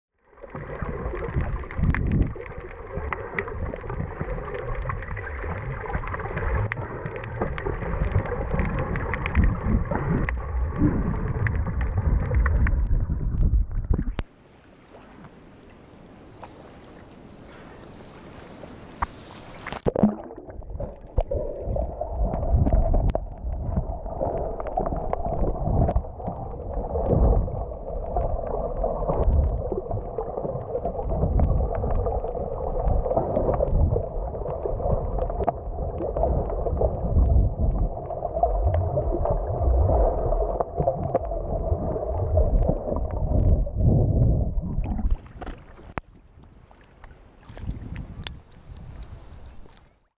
Bird diving for fish in Sydney, Australia
I found this little dude diving for fish at Shelley Beach in Manly, Sydney, Australia.. any idea what species?